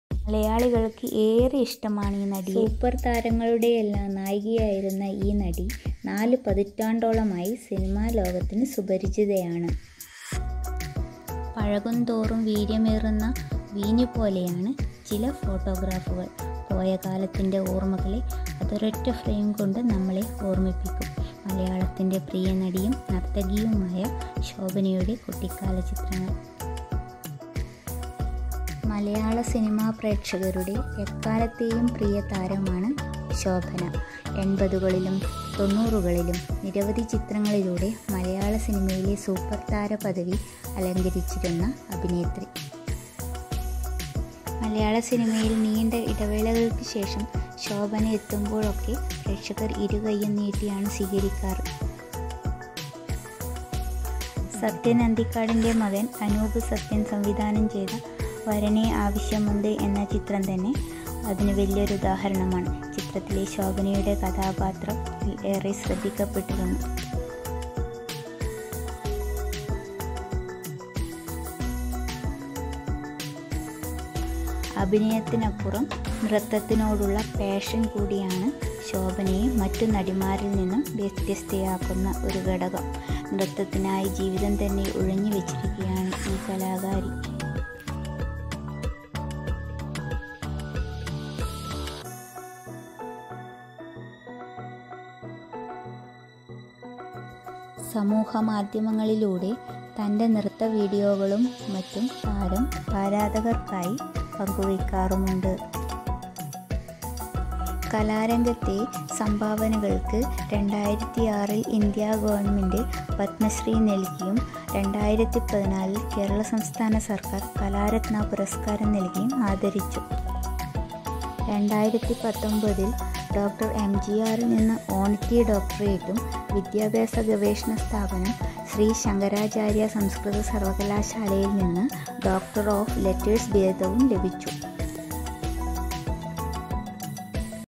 മലയാളികൾക്ക് ഏറെ പ്രിയങ്കരിയായ നടി ശോഭന ❣️എന്തോ...ഒരുപാടിഷ്ടമാണ് മലയാളികൾക്ക് ഈ നടിയെ 😍❣️shobana
സൂപ്പർതാരങ്ങളുടെയെല്ലാം നായികയായിരുന്ന ഈ നടി നാലുപതിറ്റാണ്ടോളമായി സിനിമാലോകത്തിന് സുപരിചിതയാണ്❣️shobana

This channel does not promote or encourages any illegal activities. All contents provided by this channel for GENERAL AND EDUCATIONAL PURPOSES ONLY.